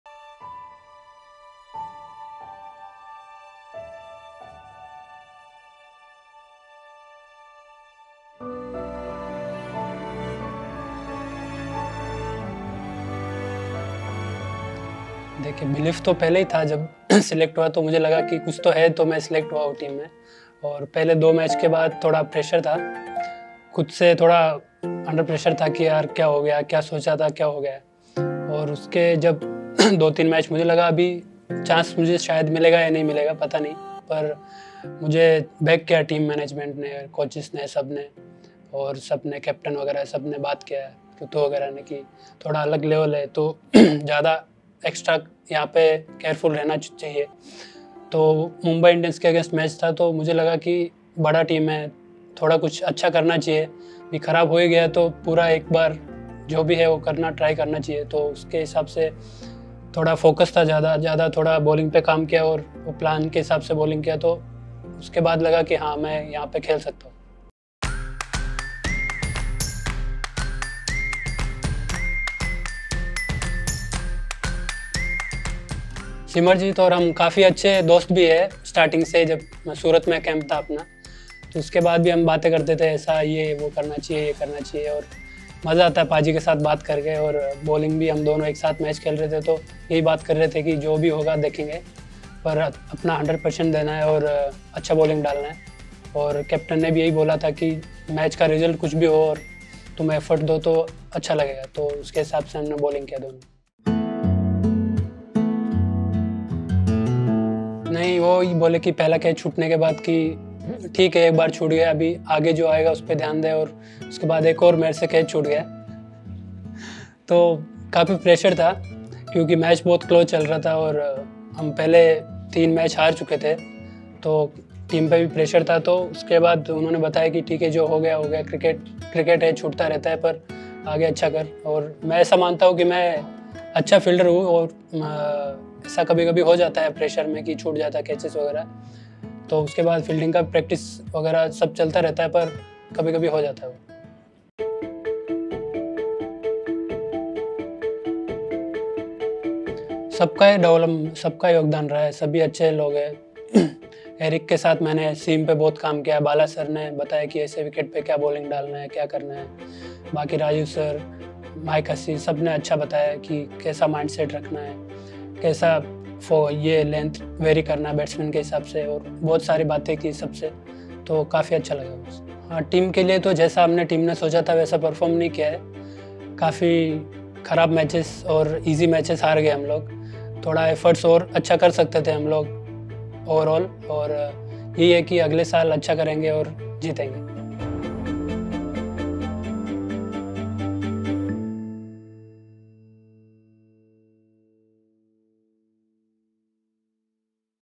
Spinning off the Summer Memories with Mukesh Choudhary | The Season that was
Mukesh takes us on a ride along the journey of his maiden Yellove season!